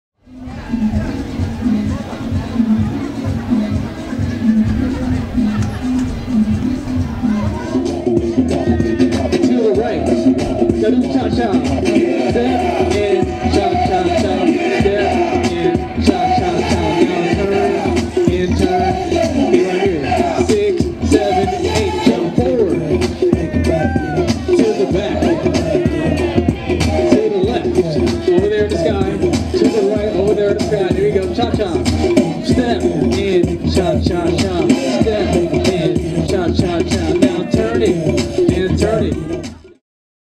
DJ Ken Teaches the Wobble to Wedding Party at Moab roof in Lancaster Ca
DJ Ken Wynn gets the crowd dancing with some great instruction!
for info on the many services we provide from DJ/MC, Video, Photo Booth, Uplighting and Photography.